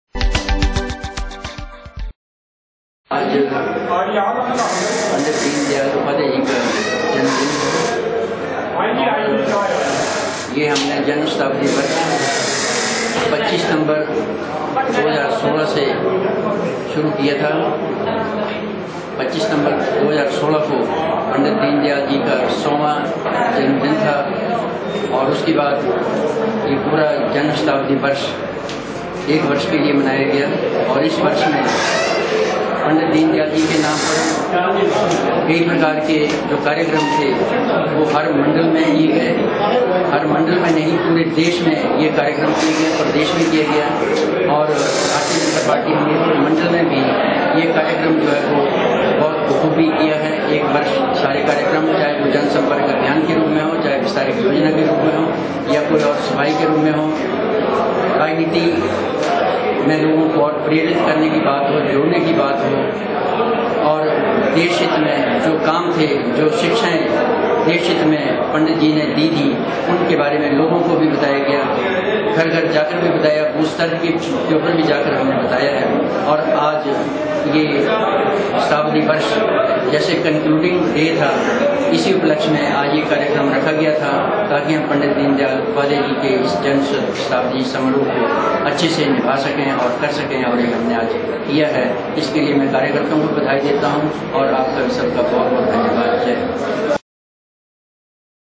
HMR BJP MANDAL पं दीन दयाल उपाध्याय के जन्म शताब्दी दिवस
पं दीन दयाल उपाध्याय के जन्म शताब्दी दिवस के अवसर पर हमीरपुर भाजपा मण्उल द्वारा कार्यकम्र का आयेजन किया गया । पं दीन दयाल उपाध्याय के सन्देश उेकर लोगो को जागरूक किया गया और उनके बताए मार्ग पर चलने के लिए आवाहन किया गया ।  हमीरपुर भाजपा मण्डल के अध्यक्ष बलदेव धीमान ने बताया कि पं दीन दयाल उपाध्याय के जन्म शताब्दी दिवस पर लोगो को घर घर जाकर जागरूक किया गया ।